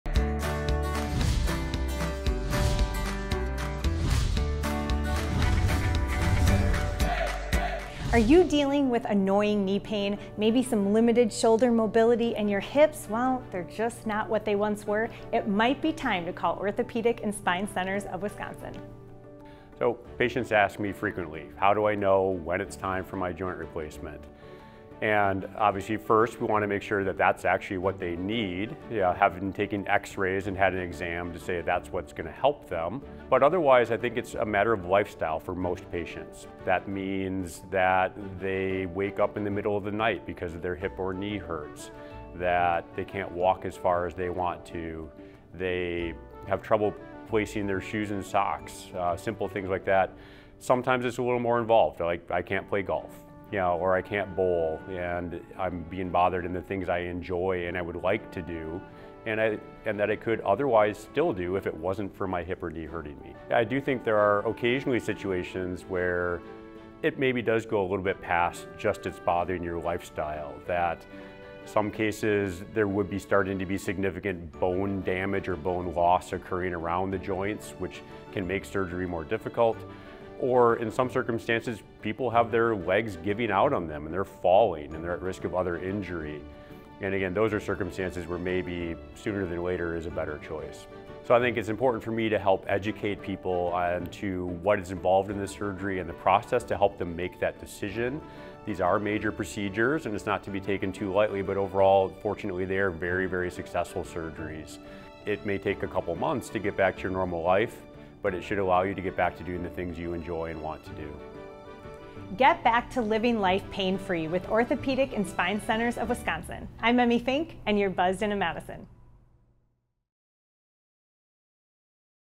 Buzzed into Orthopedic and Spine Centers of Wisconsin - Dr. Steffin
Are you bothered by nagging knee pain, restricted shoulder movement, or hip discomfort? It might be time to consult the experts at the Orthopedic and Spine Centers of Wisconsin. Join Dr. Brian Steffin as he discusses vital signs indicating it's time for joint replacement. From sleep disruptions due to pain to limitations in daily activities like walking and putting on shoes, Dr. Steffin explains the lifestyle factors that could necessitate surgery. Learn about the considerations, the process, and when it's crucial to opt for these procedures. Discover the transformative potential of these surgeries, helping you regain your passion and enjoy life to the fullest!